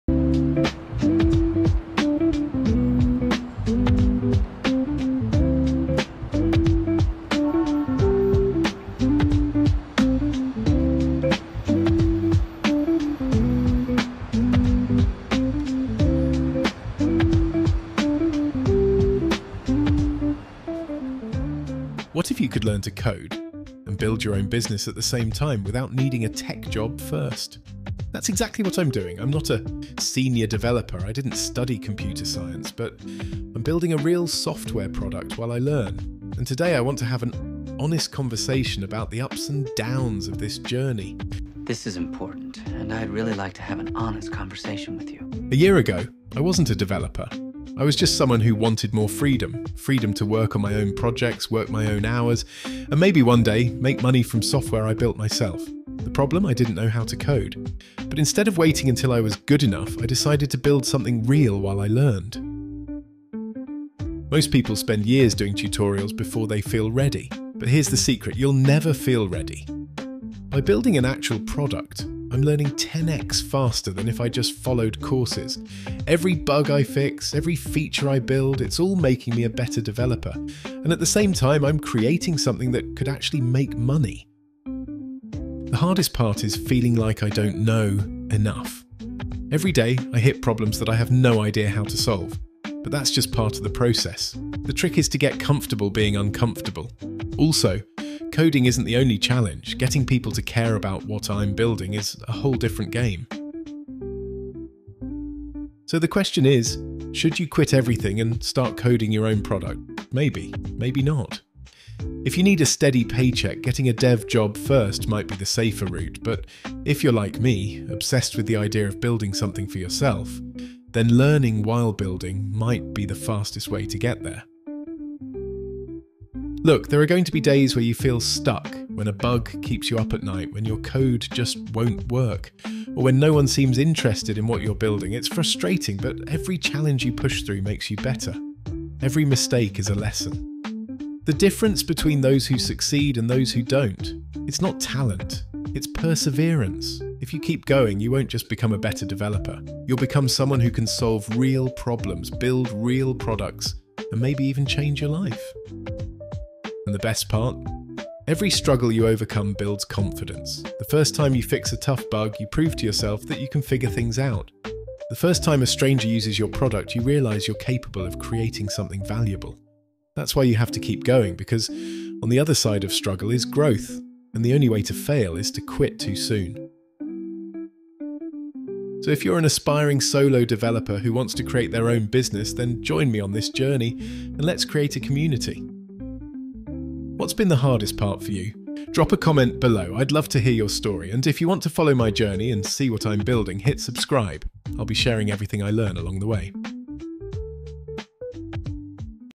Self-taught Developer Building a Startup
🚀 Learn to Code with Scrimba – My go-to platform for becoming a self-taught developer!

The past few days have been a steep learning curve—fixing bugs, building a landing page, and shifting from coding to marketing. In this video, I’ll share the experience of launching my first Chrome extension, Fast Folders, and what I’ve learned along the way.
Bear in mind this is a very early stage extension, but it feels great to actually put a product out there.

for the inspiration on this.

Plus, if you’re stuck on where to start with coding, I’ve got some advice on how to take action, stop overthinking, and start building real projects!

As a new developer, I decided to skip the traditional career path and dive straight into creating my own software—a Chrome extension—while learning to code. But is this a smart move, or a recipe for burnout? In this video, I share my honest experience, the pros and cons, and whether YOU should consider this path.

If you’re an aspiring developer, indie hacker, solo founder, or solopreneur, this video is for you. Whether you’re tired of the 9-to-5 grind or just want to build something of your own, I’ll walk you through the struggles, lessons, and wins of learning to code while working on a real project.

💡 Thinking about learning to code while building your own project? Drop a comment and share your thoughts!

Chapters:
00:00 - Intro
00:41 - Why
01:02 - Project based learning
01:25 - Imposter syndrome
01:46 - The question
02:06 - Challenges
02:38 - Overcoming struggles